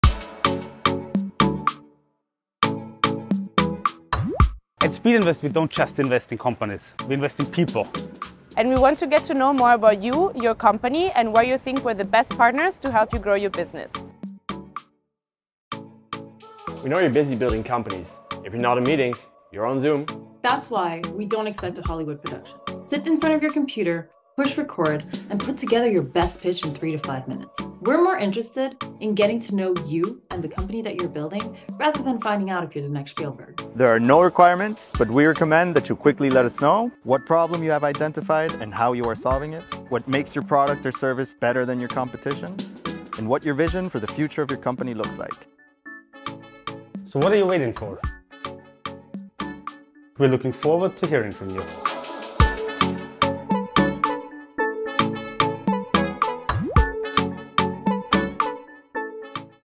How To Submit Your Startup Video Pitch to Speedinvest
Watch this brief video for simple instructions on what to include in your startup video pitch to Speedinvest.

We're looking forward to meeting you!